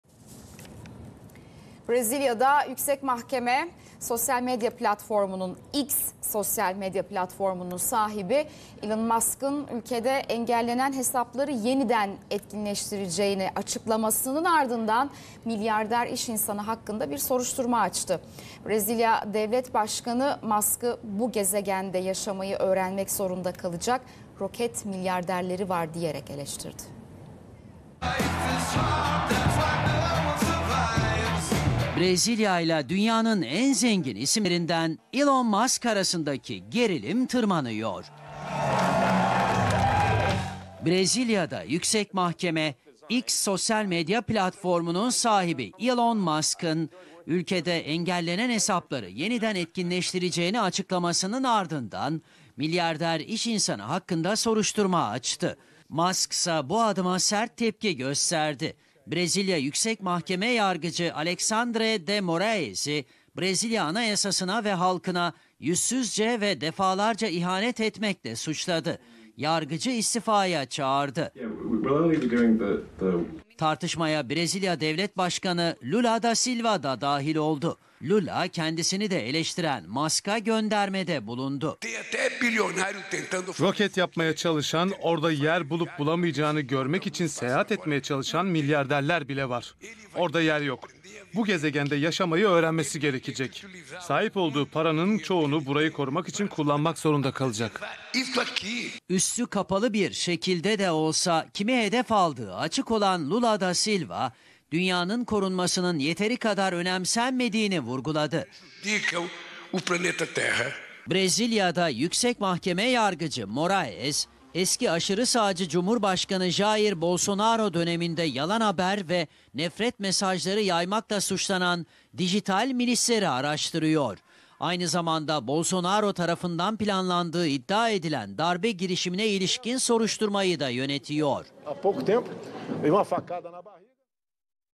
Elon Musk-Brezilya Gerilimi Tırmanıyor! "Burada Yaşamayı Öğrenmesi Gereken Milyarderler Var"
Brezilya Devlet Başkanı Lula da, Musk'ı "Bu gezegende yaşamayı öğrenmek zorunda kalacak roket milyarderleri var" diyerek eleştirdi.